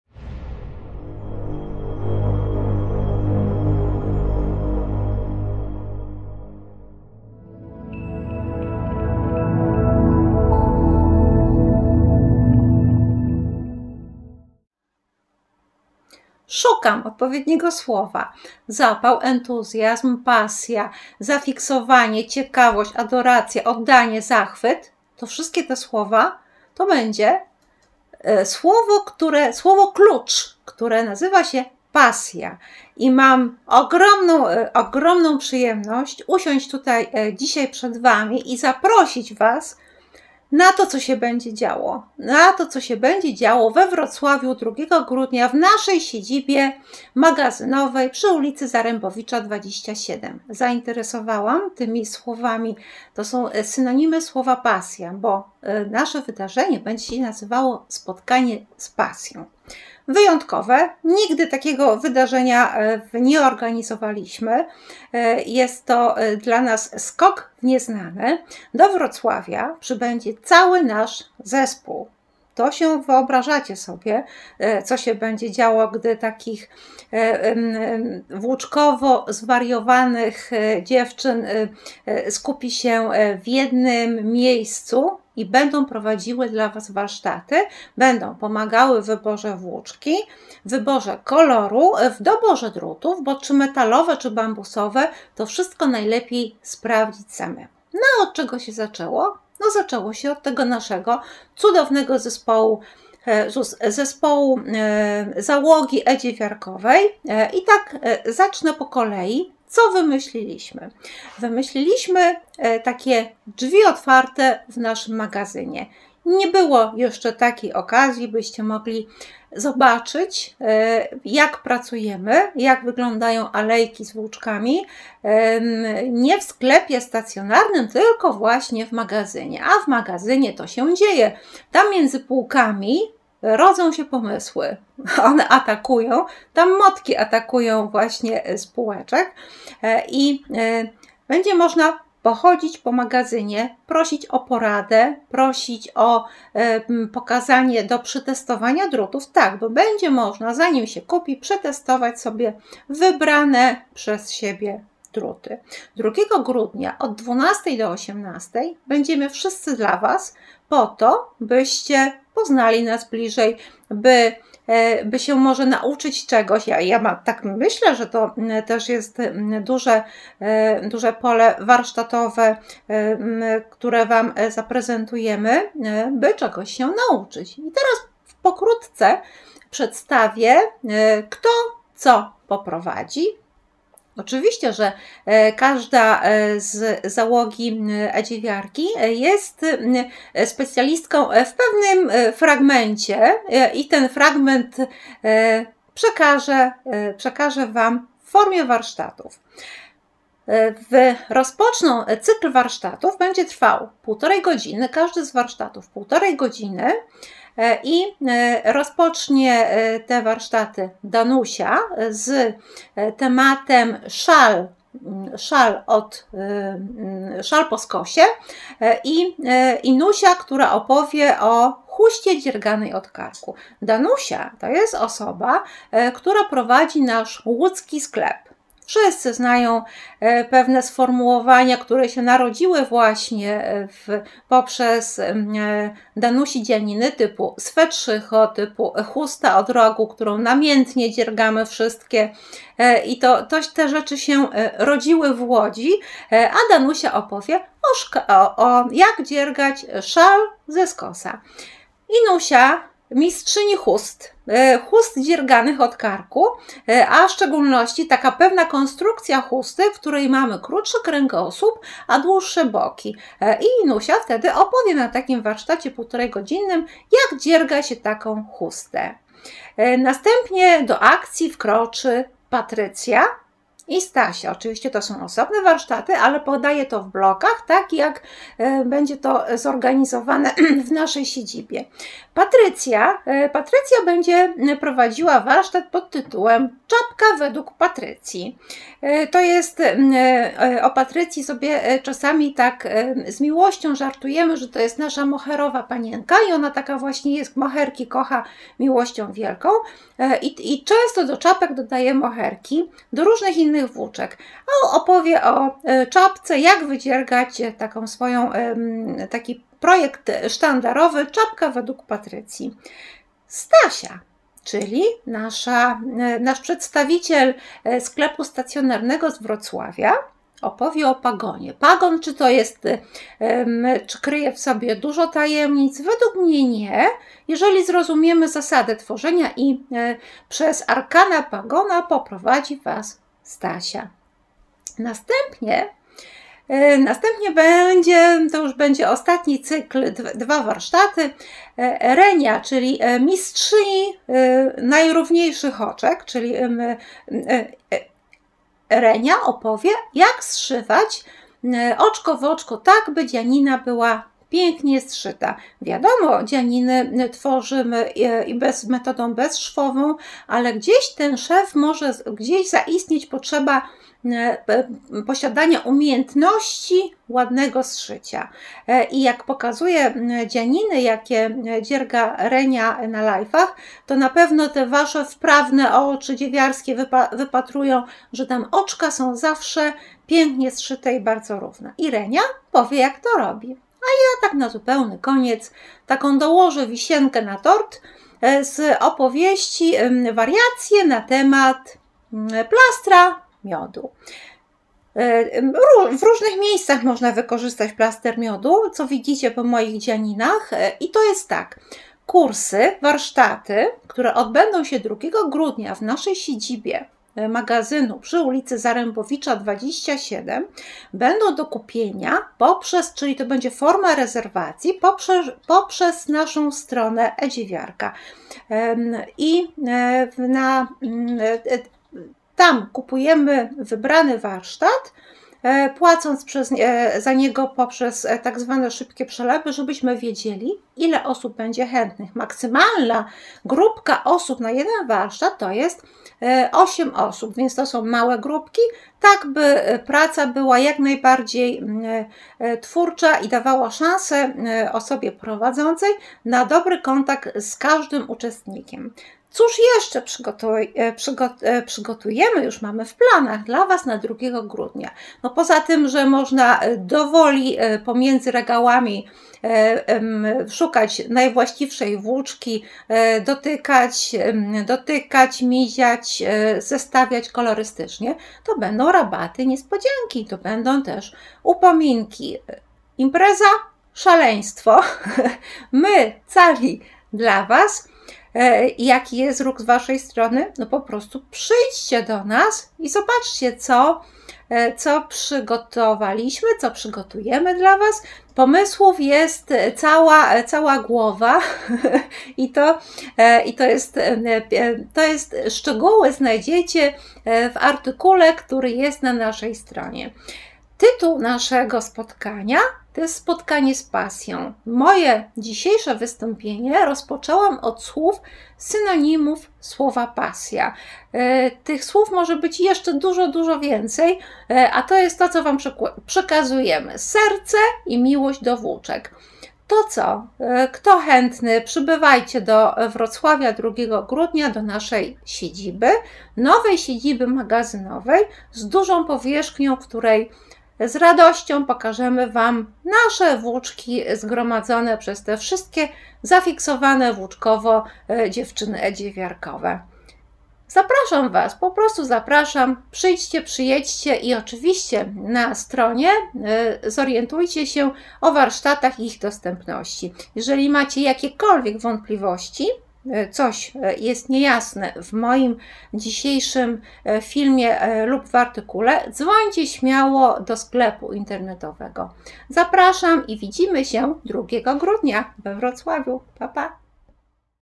Spotkanie z pasją - 135 odc. e-dziewiarka TV
Uwaga! Zmieniliśmy temat warsztatów. Zamiast czapki będą skarpety.

Spotkanie z Pasją: Warsztaty, Rabaty i Spotkanie z Zespołem!"
2 grudnia 2023 r. w godz. 12-18. Wstęp wolny.

Co powiesz na to, żeby spędzić dzień w towarzystwie pasjonatów dziergania? Jeśli tak, to mamy dla Ciebie dobrą wiadomość!
Zapraszamy Cię na Spotkanie z Pasją, które odbędzie się w magazynie sklepu e-dziewiarka.pl. To wyjątkowa okazja, żeby poznać nasz zespół, nauczyć się nowych technik i wzorów, a także kupić piękne włóczki i akcesoria z rabatem.

Nie przegap tej szansy i zarezerwuj swoje miejsce już dziś! Spotkanie z Pasją to nie tylko zakupy, ale przede wszystkim wspólna zabawa i wymiana doświadczeń.

Będziesz mógł/mogła wziąć udział w warsztatach dziewiarskich prowadzonych przez naszych ekspertów, dotknąć naszych włóczek, wypróbować druty dziewiarskie i zapytać o wszystko, co Cię interesuje.
A na koniec spotkania czeka na Ciebie niespodzianka - prezent od e-dziewiarka.pl!

Nie czekaj dłużej i dołącz do naszego Spotkania z Pasją. Zapewniamy Cię, że będzie to niezapomniane przeżycie dla każdego miłośnika dziergania!

Plan Spotkania:
Adres:
Ul. Władysława Zarembowicza 27, 54-530 Wrocław (obok Cargo lotniska we Wrocławiu).
Spotkanie w dniu 2 grudnia 2023 r. w godz. 12-18.

Warsztaty na zapisy. Grupy do 8 osób, koszt 35 zł.:
Godz. 12:30 - 14:00
'"Chusta robiona od karku", prowadzi Inna
"Szal po skosie" prowadzi Danusia

Godz. 14:15 - 15:45
"Skarpety zaczynane od palców", prowadzi Patrycja
"Pagon", prowadzi Stasia

Godz. 16:00 – 17:30
"Zszywanie oczka w oczko" prowadzi Renata
"Wzór plastra miodu – wariacje wg Mariolci" - prowadzi Mariolcia

Aby zapisać się na warsztaty, należy wykupić uczestnictwo w danym temacie warsztatów na naszej stronie: link.

Na Spotkaniu z Pasją:
- Możesz obejrzeć dzianiny pokazywane w transmisjach na żywo na YouTube.
- Poznać ekipę e-dziewiarka.pl
- Dotknąć wszystkich włóczek i wypróbować akcesoria dziewiarskie.
- Zrobić zakupy z atrakcyjnym rabatem, korzystając z naszych porad.
- Do zakupów, atrakcyjny upominek.